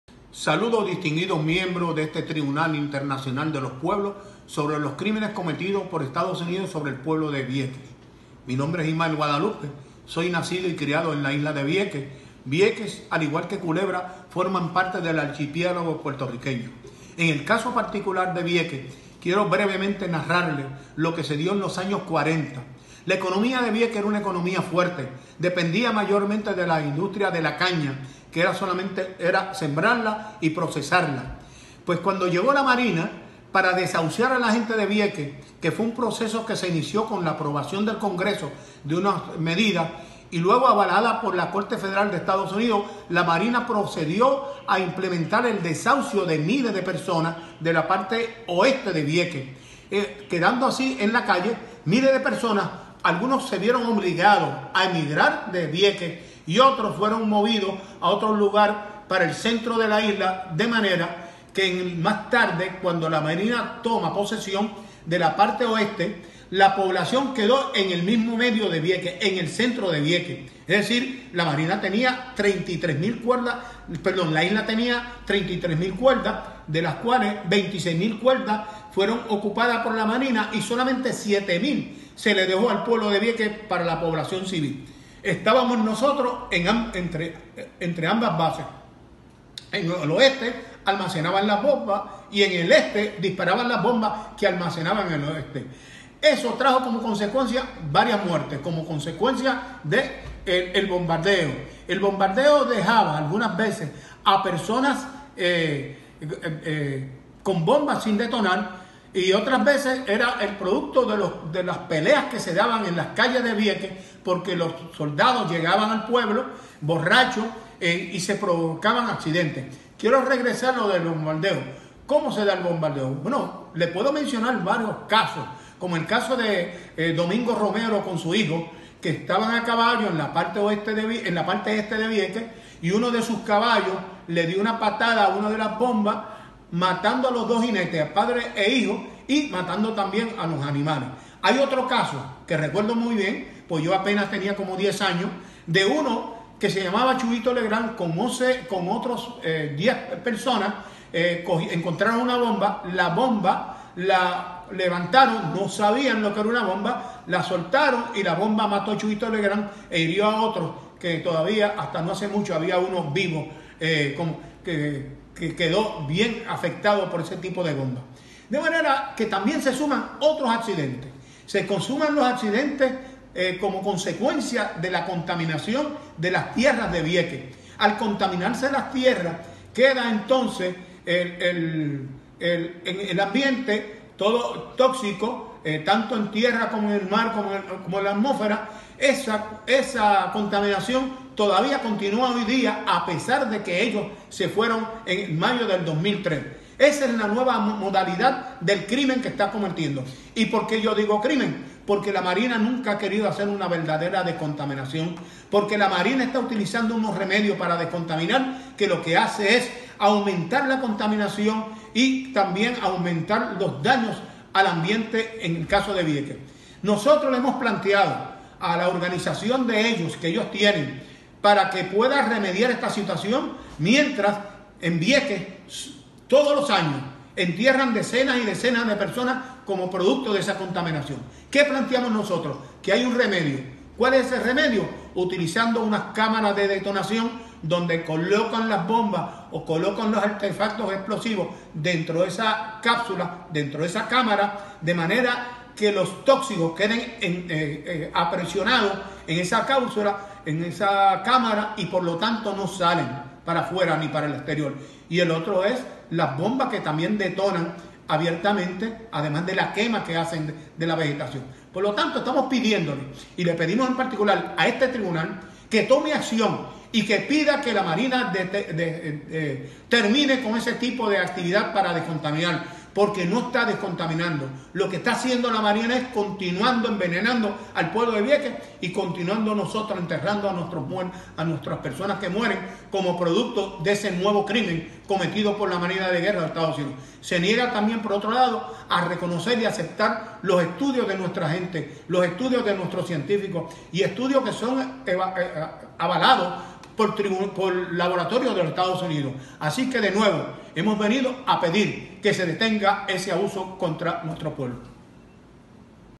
Ismael Guadalupe on Vieques Contamination
Describes the crimes of the U.S. Navy against the people of Vieques.